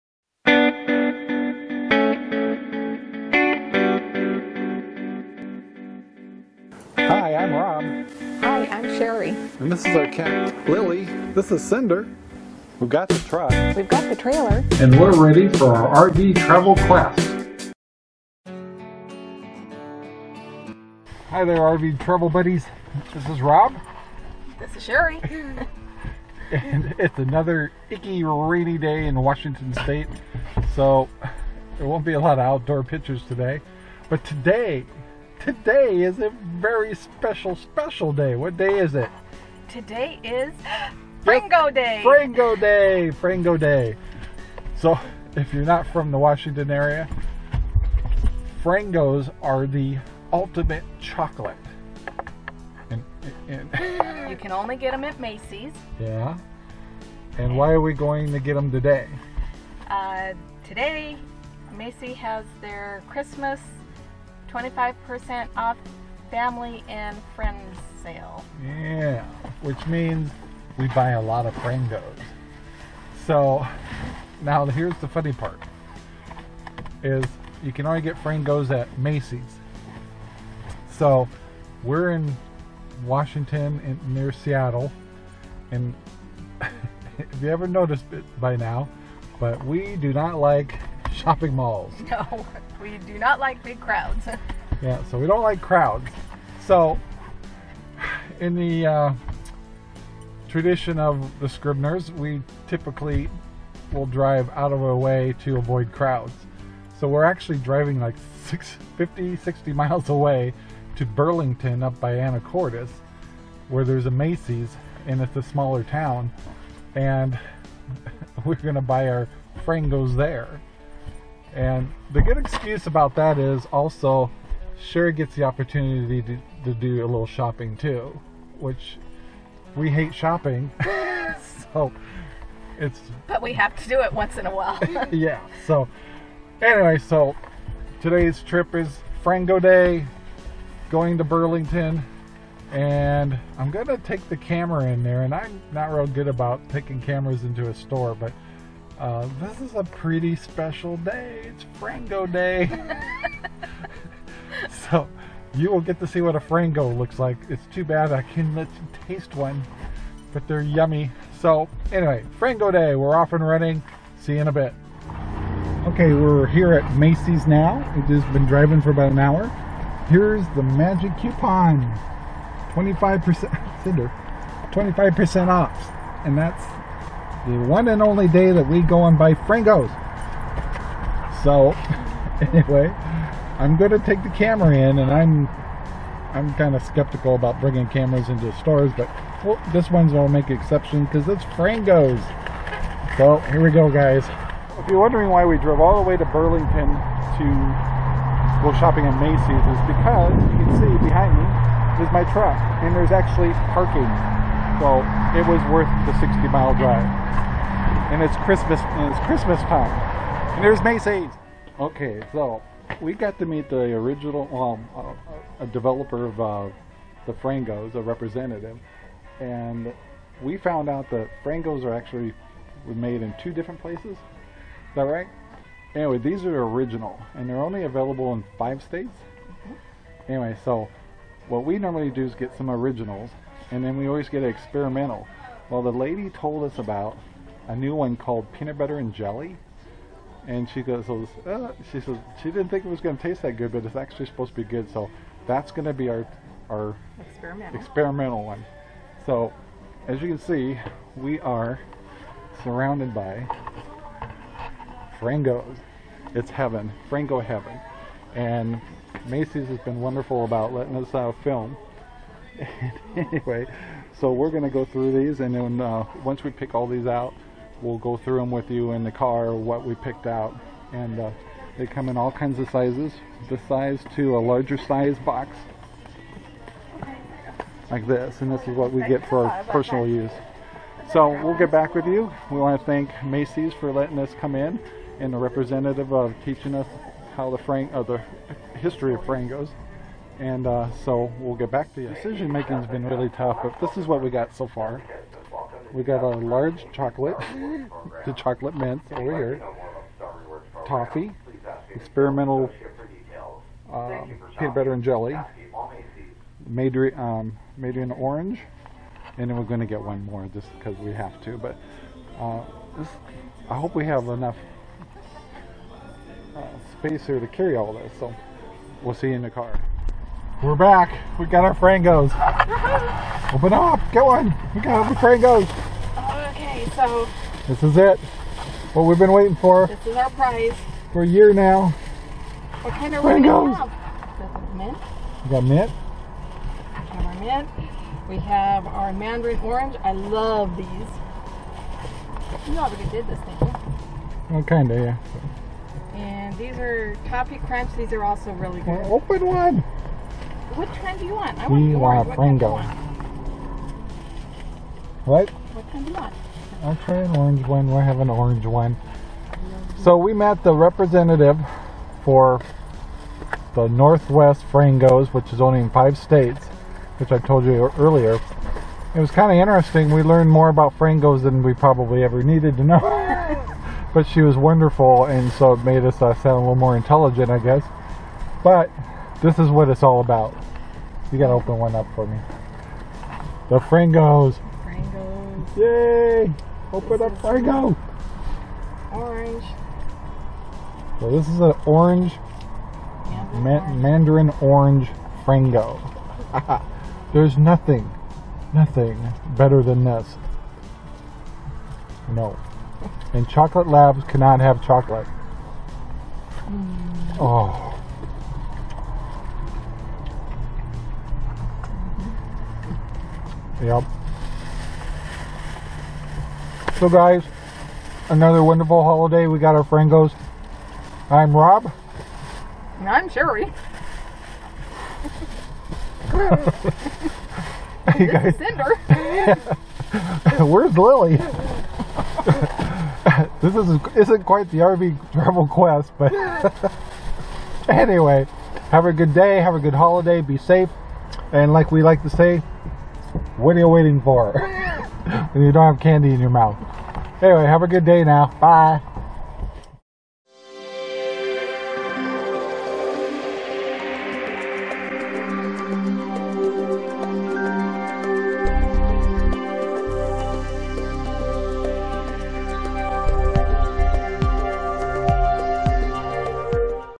It's Frangos at Macys Day Tradition | RV Fun | RV Travel Quest with Rob & Sherry
We met the Frango Representative and learned a lot about our favorite chocolate.
Join us for a trip to Birlington Washington Macys.